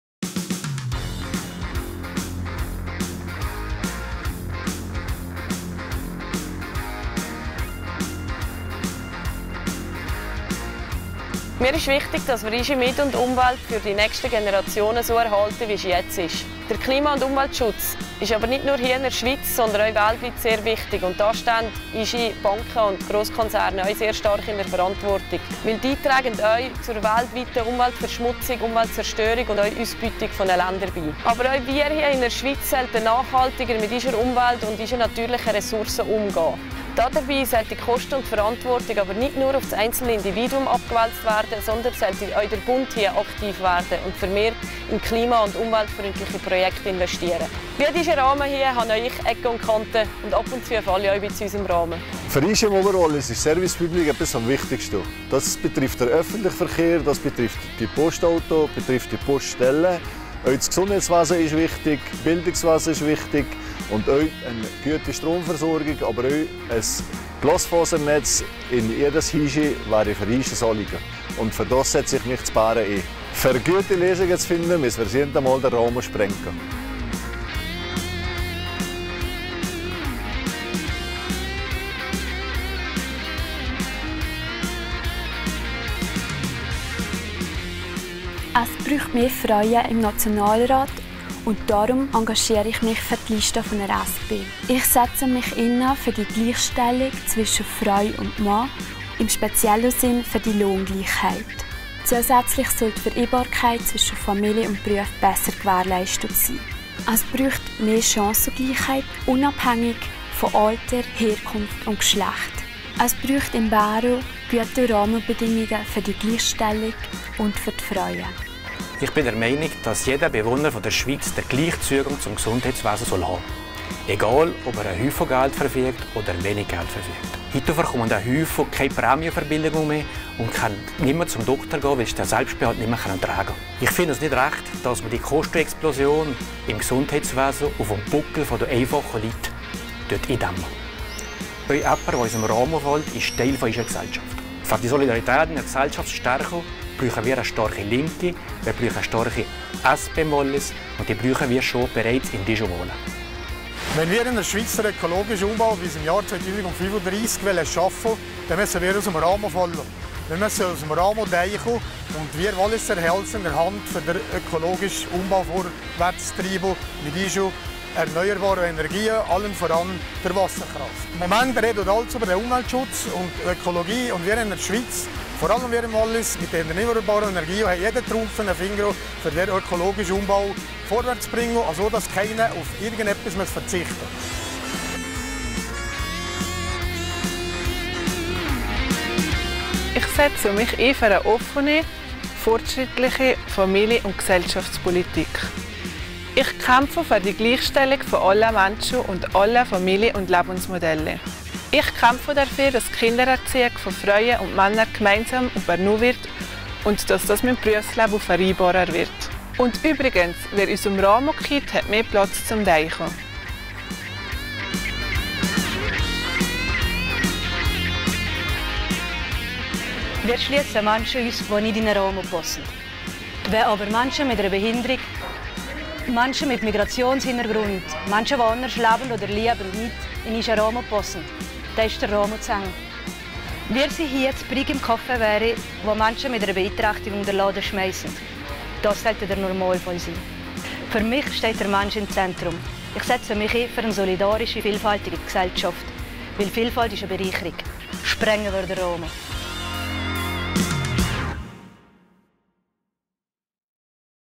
SP Oberwallis Wahlclip Nationalratswahlen 2019
Wahlclip mit allen sieben KandidatInnen